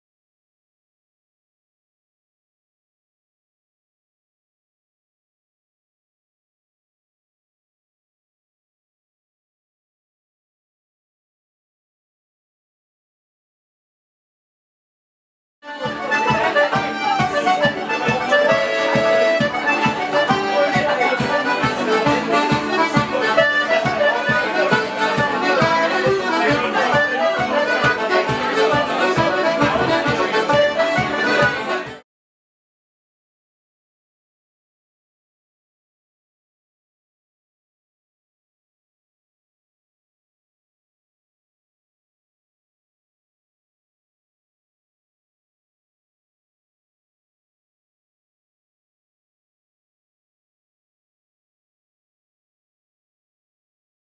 Geese in the Bog, Kinsale, Ireland
Geese in the Bog is an Irish musical group from Kinsale, Ireland.  Photography and video from James Gordon Patterson, Phoenix, Arizona.